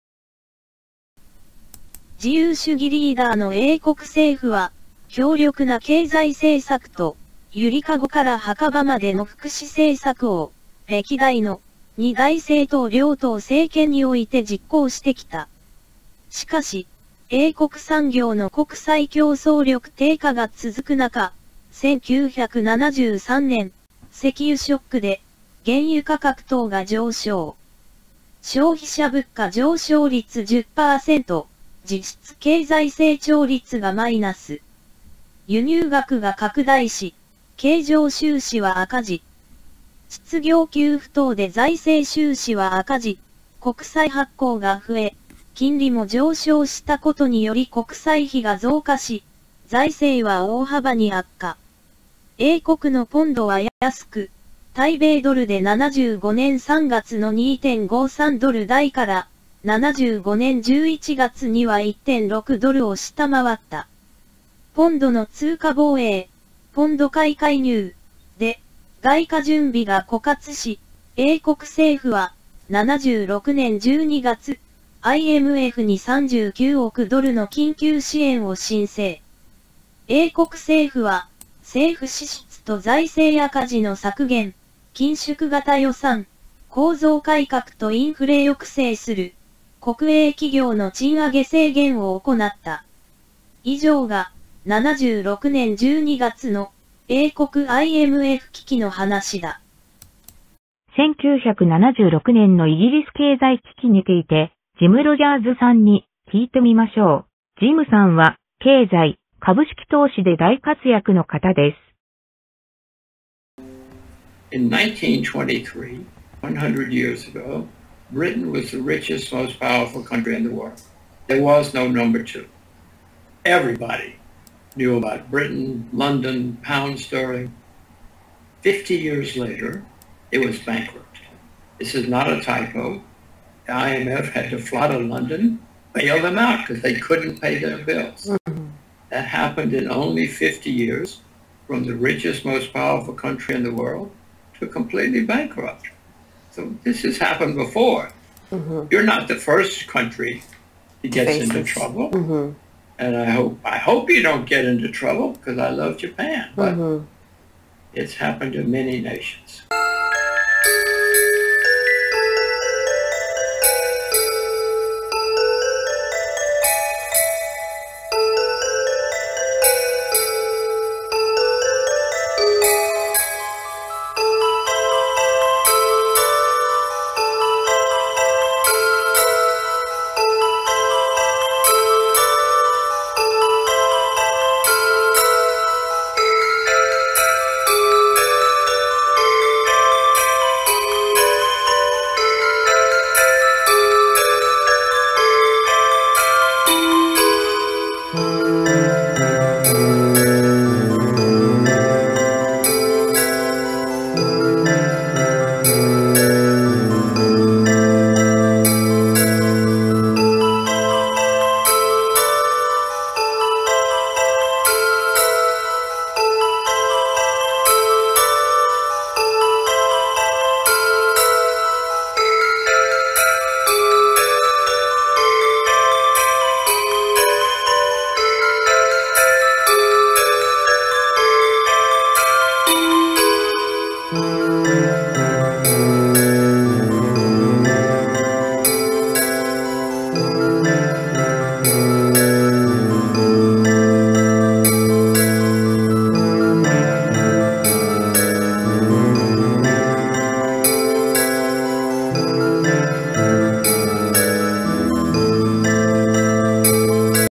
世界大投資家,ジム・ロジャーズ氏,英国ＩＭＦ危機[1976年12月]を語る。
引用：内閣府「英国ＩＭＦ危機(76年)」2020年。楽待 RAKUMACHI 【巨大バブルの崩壊】"世界三大投資家"が「日本終了」を予測する理由。